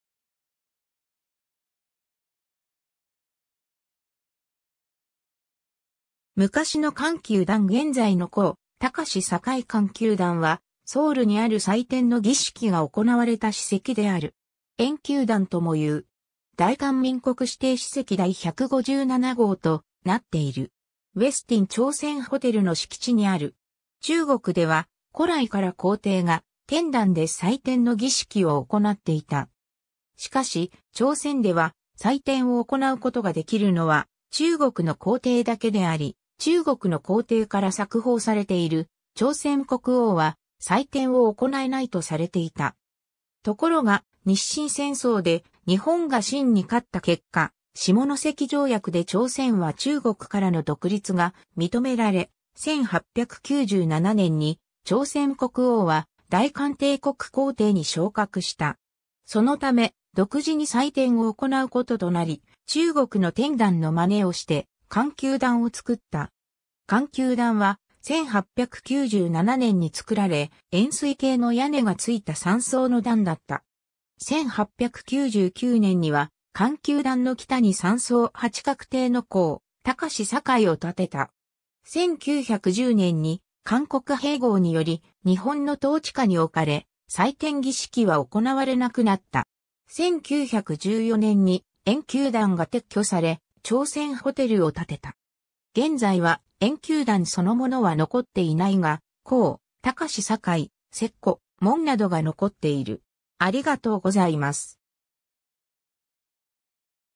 圜丘壇
中区_(ソウル特別市)
昔の圜丘壇 現在の皇穹宇 圜丘壇（えんきゅうだん）は、ソウルにある祭天の儀式が行われた史跡である。
円丘壇（원구단、ウォングダン）ともいう。
大韓民国指定史跡第157号となっている。
ウェスティン朝鮮ホテルの敷地にある。
 中国では、古来から皇帝が天壇で祭天の儀式を行っていた。
しかし、朝鮮では、祭天を行う事ができるのは中国の皇帝だけであり、中国の皇帝から冊封されている朝鮮国王は、祭天を行えないとされていた。
ところが、日清戦争で日本が清に勝った結果、下関条約で朝鮮は中国からの独立が認められ、1897年に朝鮮国王は大韓帝国皇帝に昇格した。
そのため、独自に祭天を行うこととなり、中国の天壇のまねをして圜丘壇を作った。
 圜丘壇は、1897年に作られ、円錐形の屋根が付いた3層の壇だった。
1899年には、圜丘壇の北に3層8角亭の皇穹宇（황궁우、ホァングンウ）を建てた。
1910年に韓国併合により日本の統治下に置かれ、祭天儀式は行われなくなった。
1914年に円丘壇が撤去され、朝鮮ホテルを建てた。
 現在は、円丘壇そのものは残っていないが、皇穹宇、石鼓（석고、ソッコ）、門などが残っている。